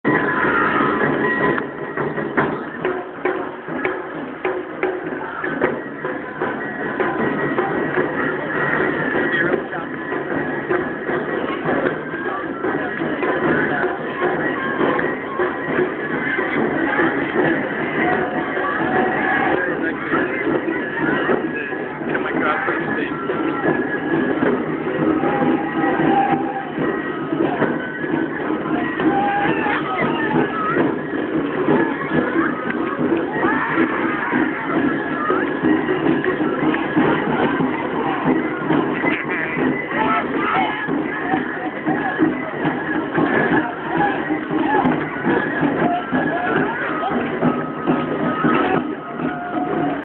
Kagurazaka Matsuri 3
Kagurazaka Matsuri in Tokyo July 27 2007
This is a cellphone video and doesn't scale up from tiny very well. Best viewed with a squint after a few beers.